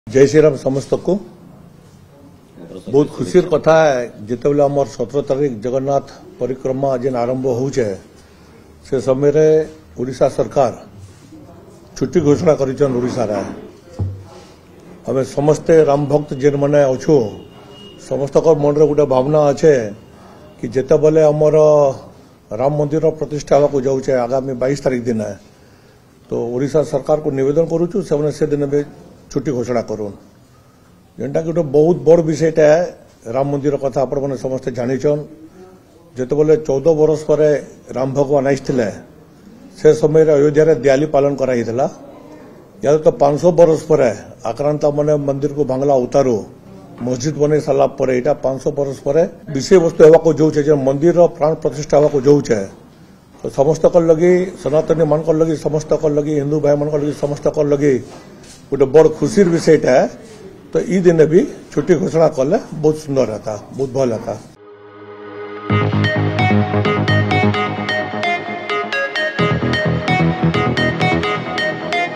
’17ରେ ଛୁଟି 22ରେ ବି ହେଉ’ | BJP's Girish Patel Urges Holiday for Ram Temple Event | Odisha Reporter
ଆଗାମୀ 22ରେ ରାମ ମନ୍ଦିର ପ୍ରତିଷ୍ଠା ହେବାକୁ ଯାଉଛି । ସେହିଦିନ ଓଡ଼ିଶା ସରକାର ଛୁଟି ଘୋଷଣା କରନ୍ତୁ :  ଗିରିଶ ପଟେଲ,  ବିଜେପି ନେତା